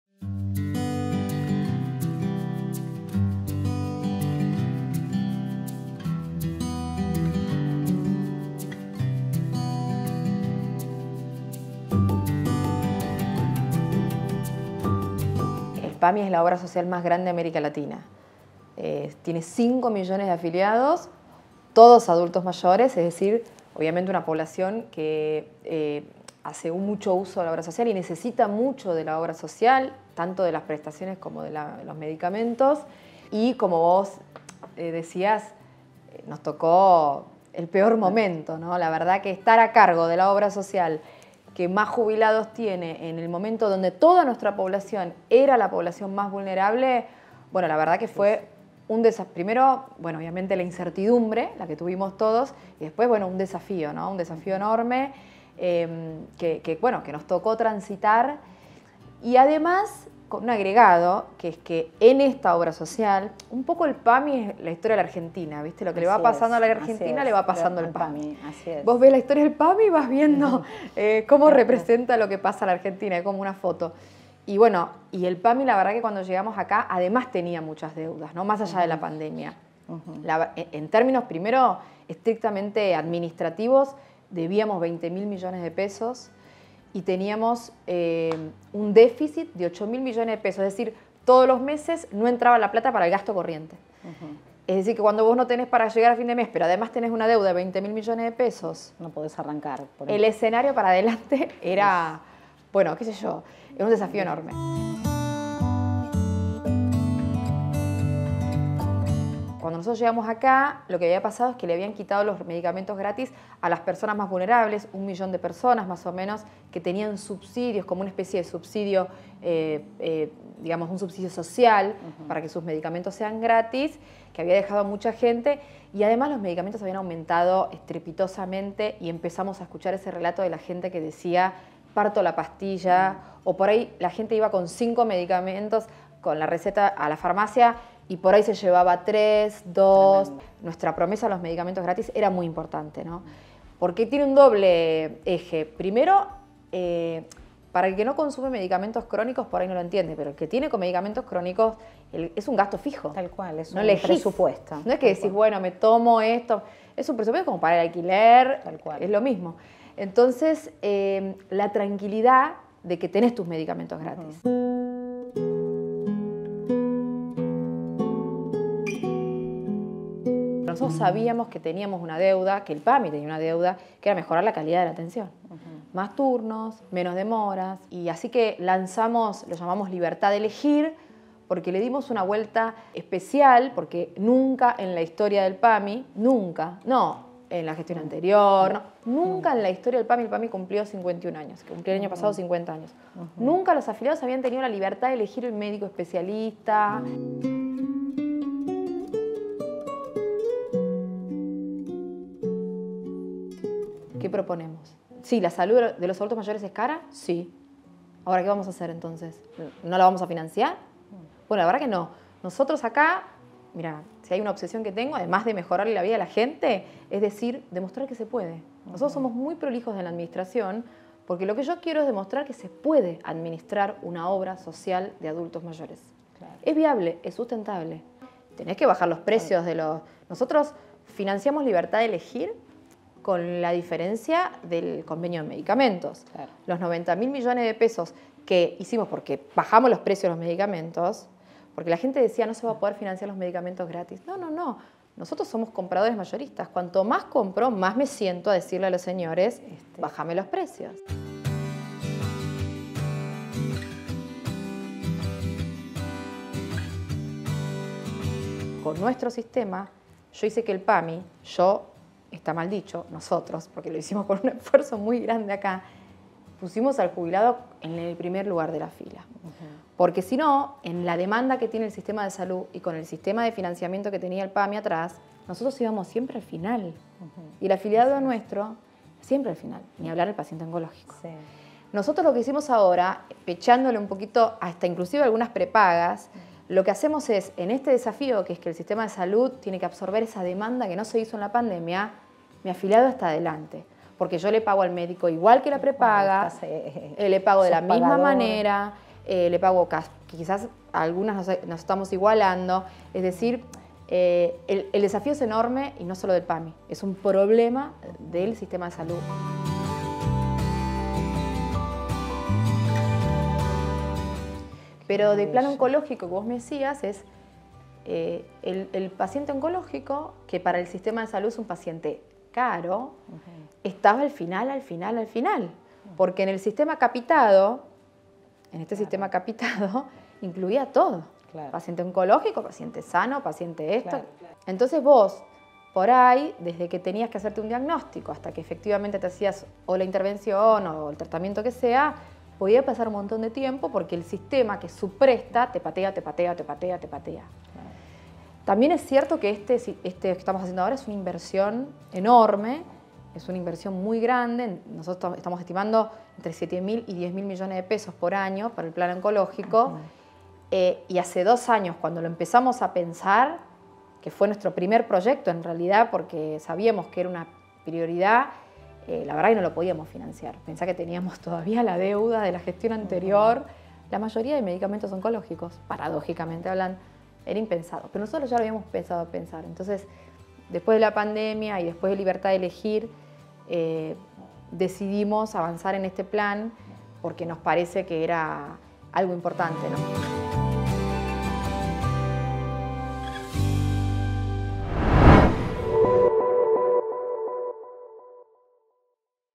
Volnovich repasa su gestión: “Los vaivenes del PAMI son un reflejo de la historia de la Argentina”
La directora ejecutiva de PAMI dialogó sin concesiones con Infobae para analizar los proyectos que encaró luego de que la pandemia por COVID castigara duramente al segmento etario de los adultos mayores. Cómo apuesta a refundar la obra social de jubilados más grande de América Latina.